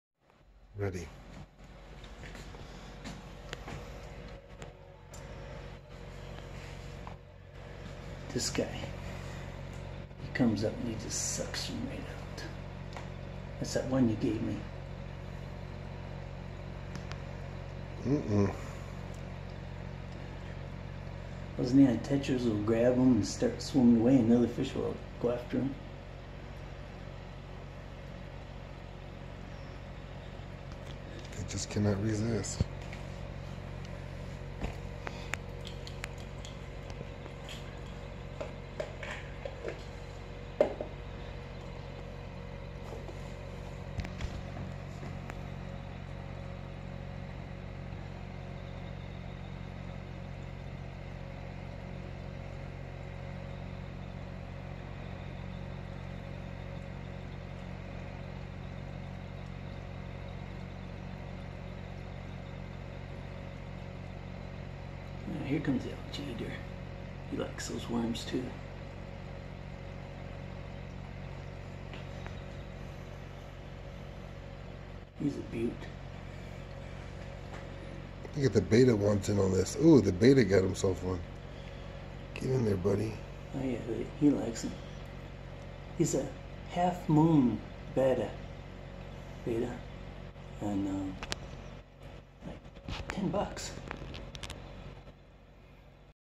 Feeding fish live worms
Community tank feeding on live worms.